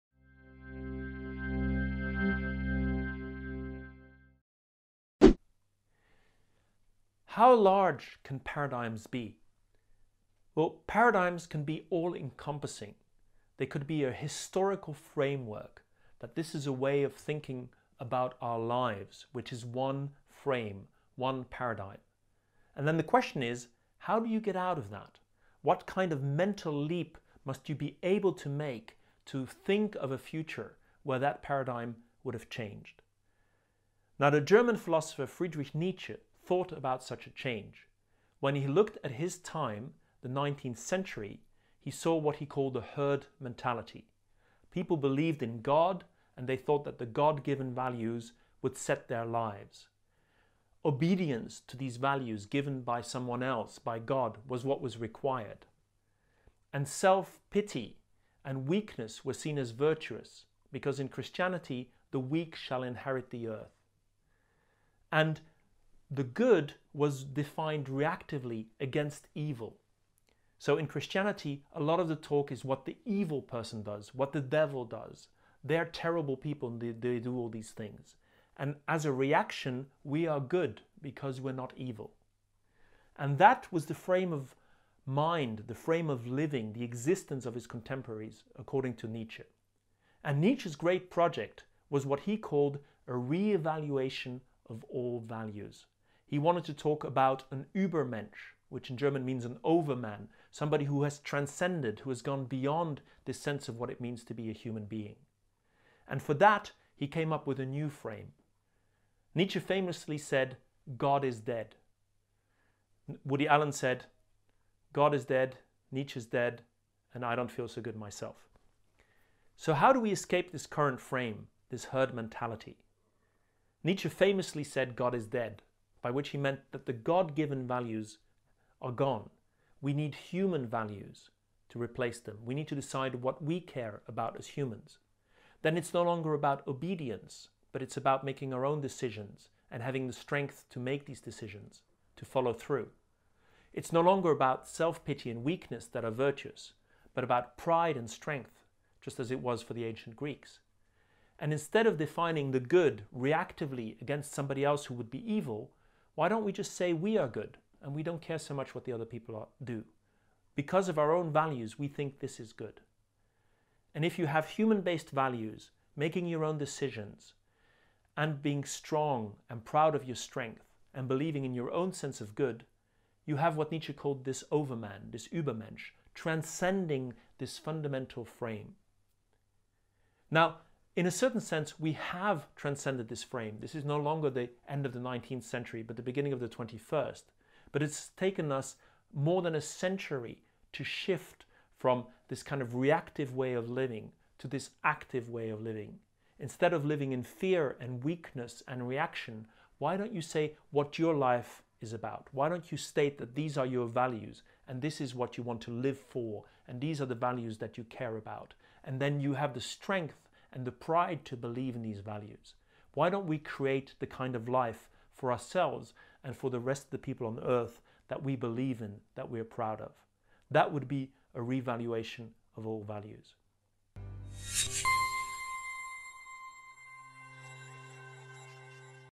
Nietzsche’s revaluation of all values​
Designing your own life and making your own choices. The philosopher Friedrich Nietzsche describes a "revaluation of all values".